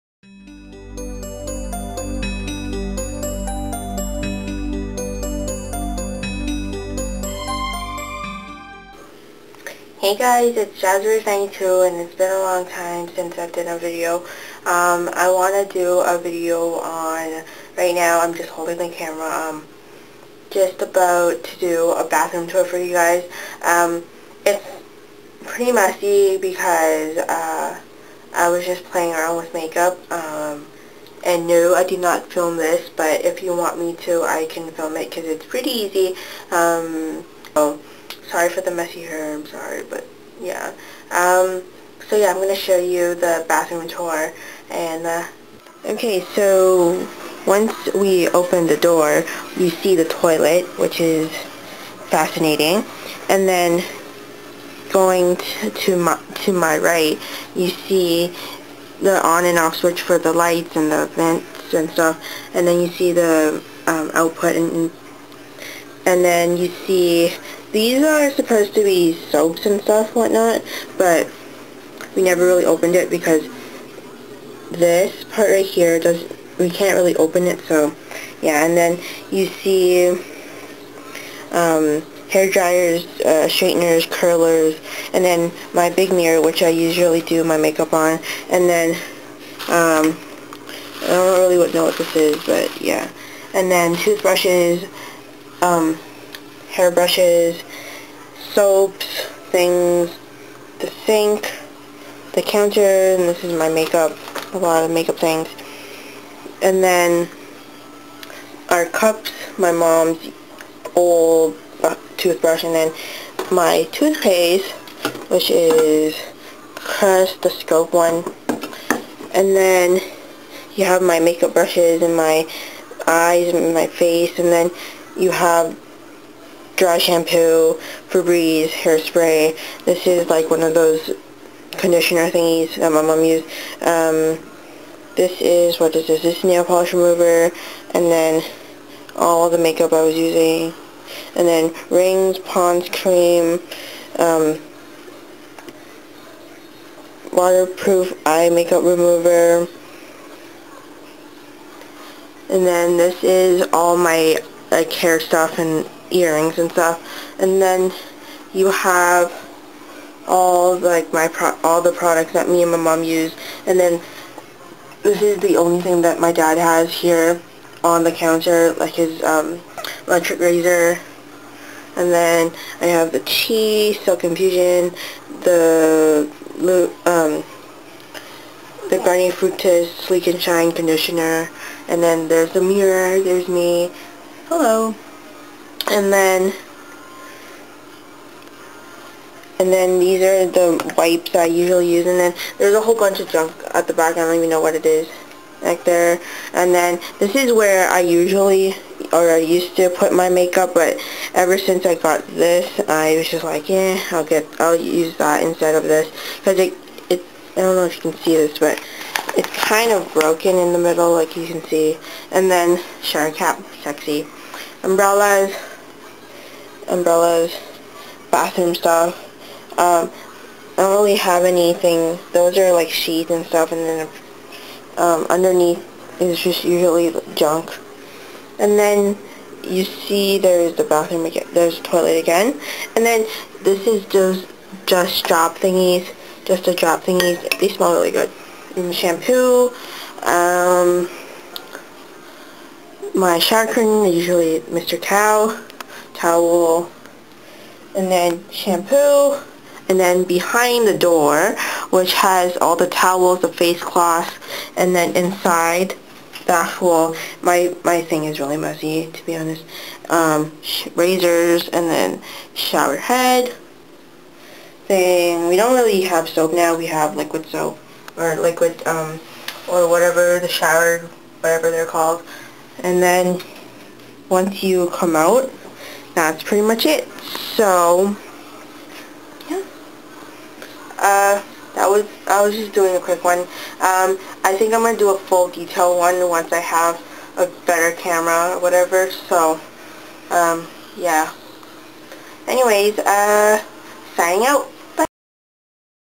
Quick Bathroom Tour~
READ HERE FIRST!!!~

hey guys..
this video i didnt intend to upload it but since
it was just a quick one i thought might as well UPLOAD it cus
i havent been uploading anything so yeaa
Love,
JazzBerries92

p.s. someone did make a request of a bathroom tour but this will not be the actual tour since it was not detailed and it wasnt in the best video quality.